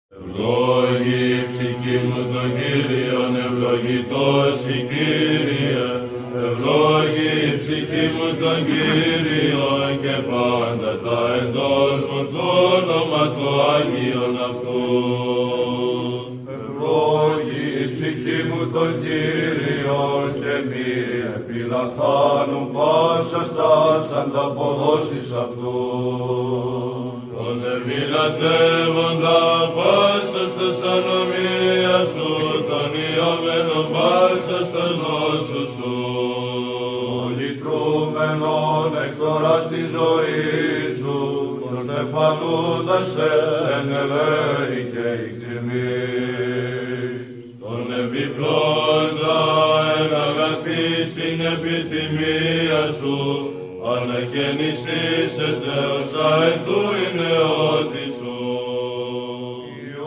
Άγιον Όρος το 1963!
Άγιον Όρος το 1963! Σπάνια πλάνα που κατέγραψε συνεργείο Γαλλικού καναλιού.